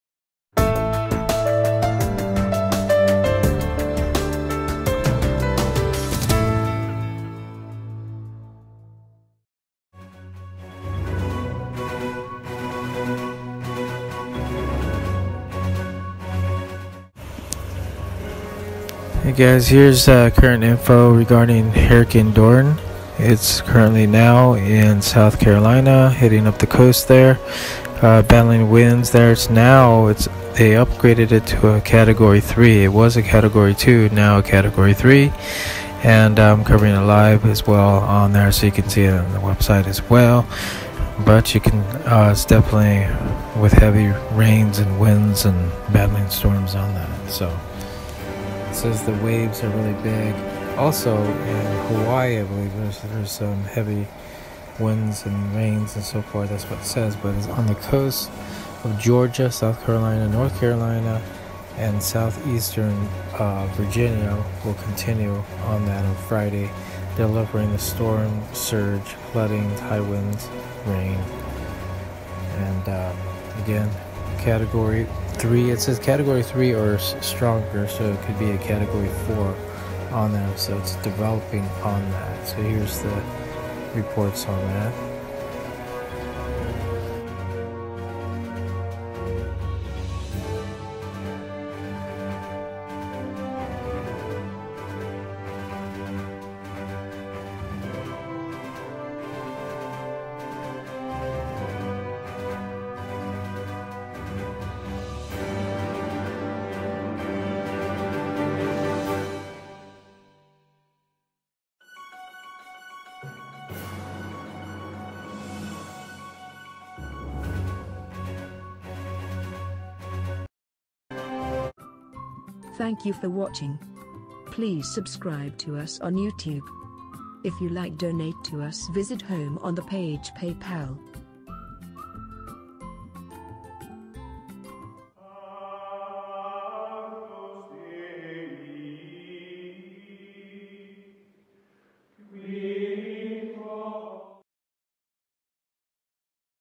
Hurricane Dorian has intensified Category 3 Carolinas
Hurricane Dorian has intensified Category 3 Carolinas major hurricane once again and will track near the coasts of Georgia, South Carolina, North Carolina and southeastern Virginia